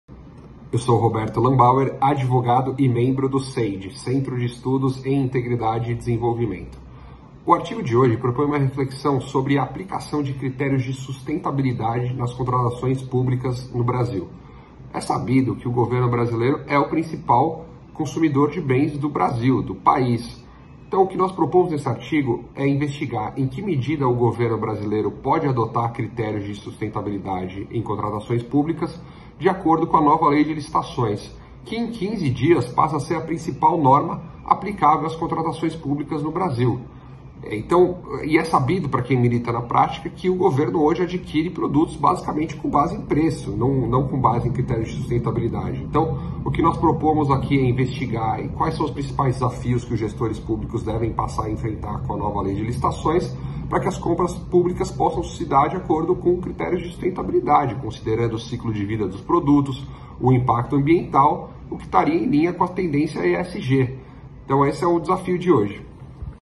Colunistas SBT News - Roberto Lambauer | A lei de licitações e equilíbrio nas contratações públicas
Publicada em 2021, a Nova Lei de Licitações (NLCC), Lei Federal nº 14.133, está em seus últimos ensaios para se tornar obrigatória em toda a Administração em dezembro deste ano. Foram duas prorrogações de prazo de vigência, refletindo a já conhecida dificuldade do Poder Público em operacionalizar mudanças legislativas estruturais. Como reflexo disso, o Tribunal de Contas da União (TCU) identificou recentemente a baixa utilização da nova lei nos certames